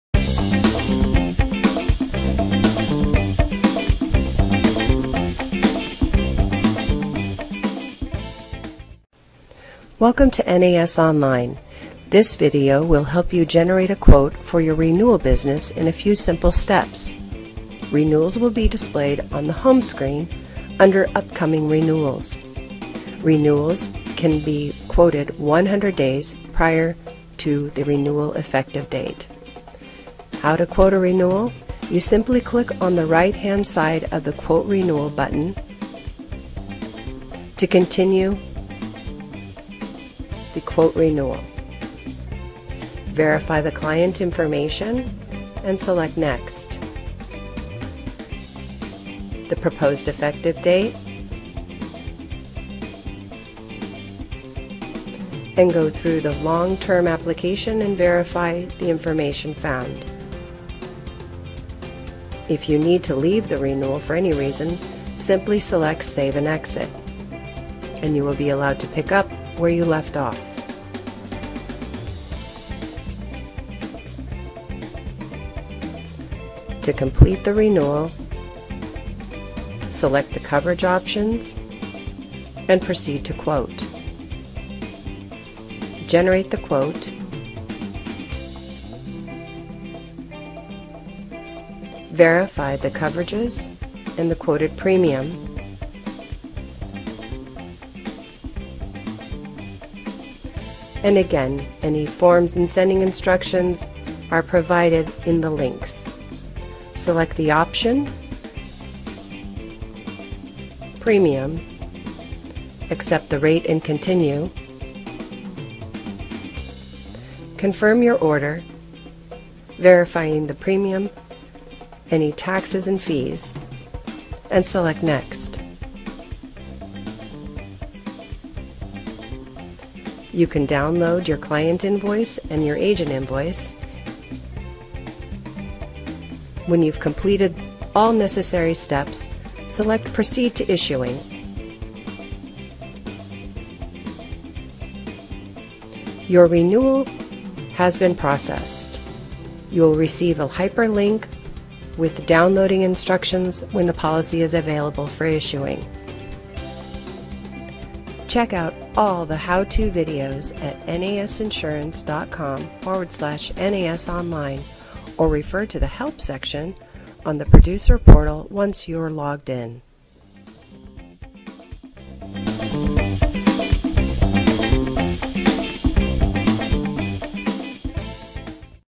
NAS Online - How To Create A Renewal Application
In this tutorial, you will learn how to use NAS Online to access existing policies in order to process a renewal for your client.